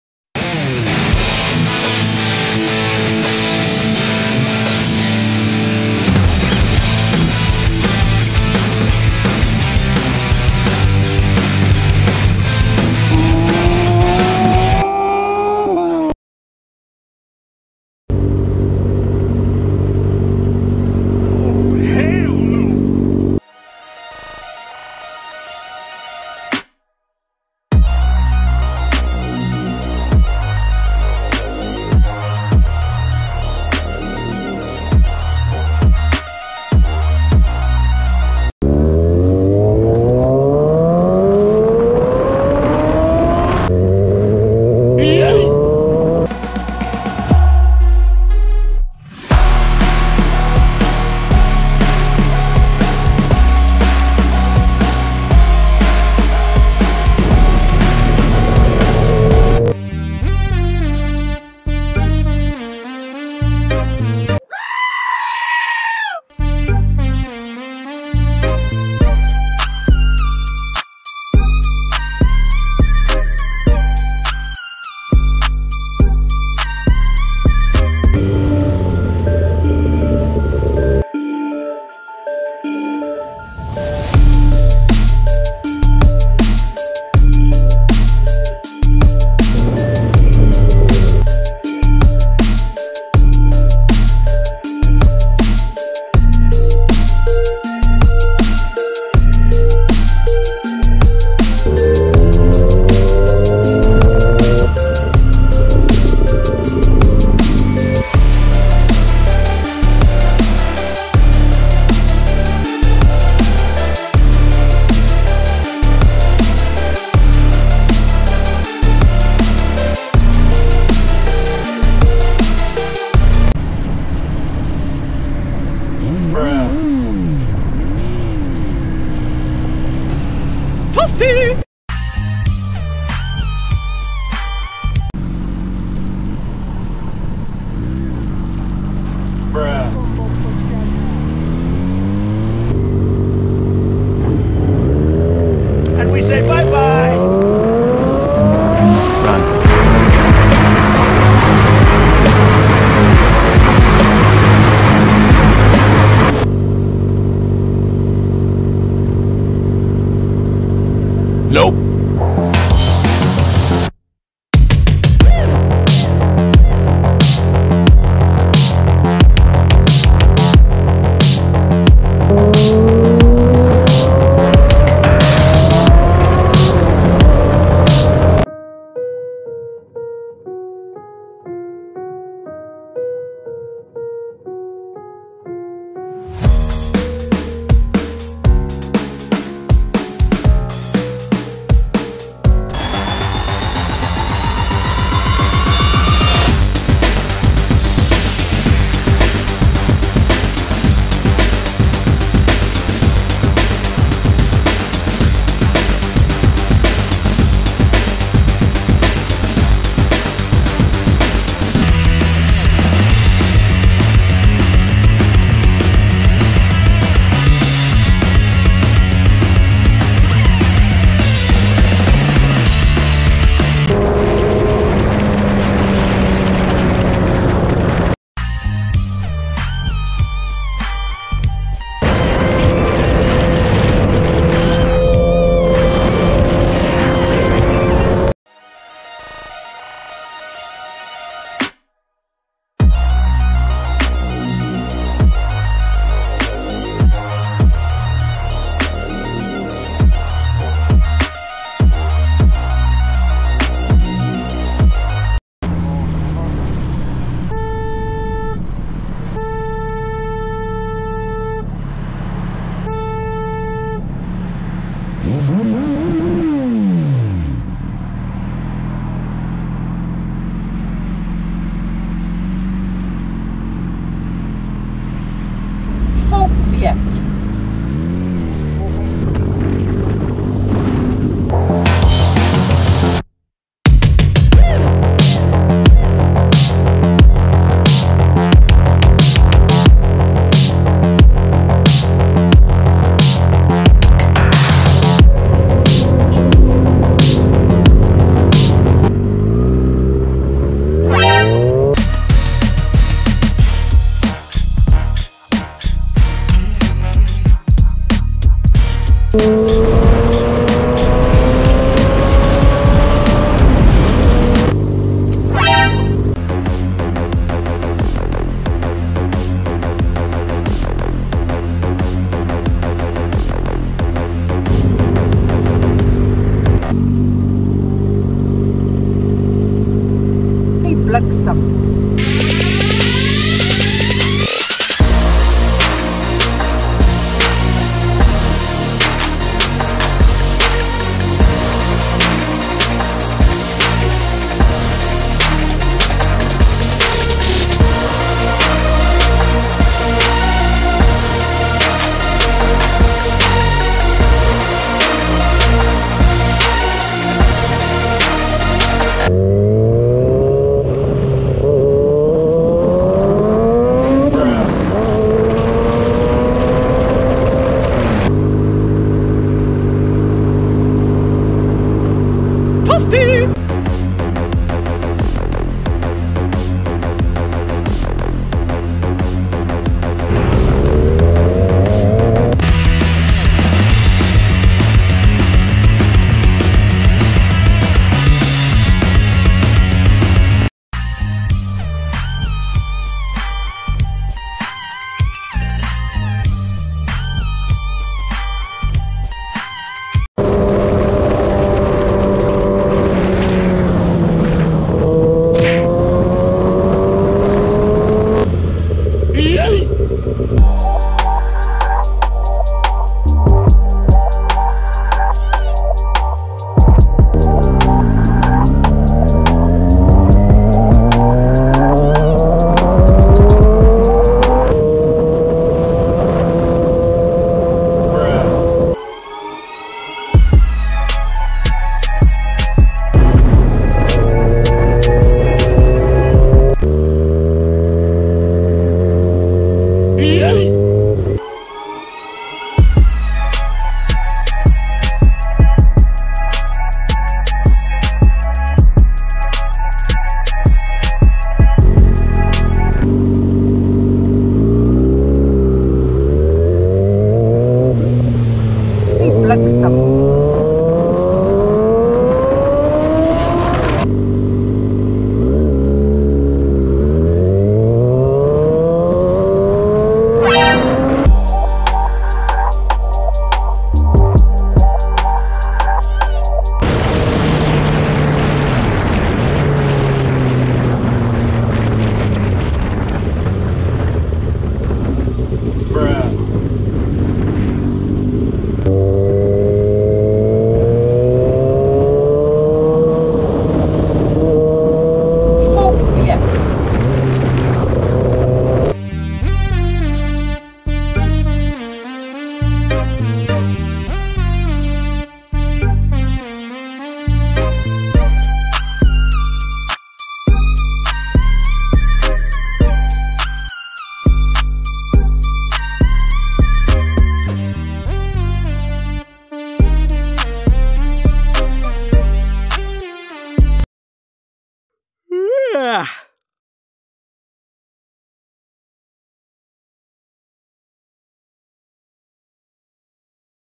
Idiot Drivers of the Week #59 (South African Bad Drivers)
Random traffic incidents for the week (South Africa, Taxi Mageza, Close Calls, Idiot Drivers, Road Rage, Load Shedding).

📢 Shoutout to Featured Youtubers who supplied clips! Show your support and give them a SUB!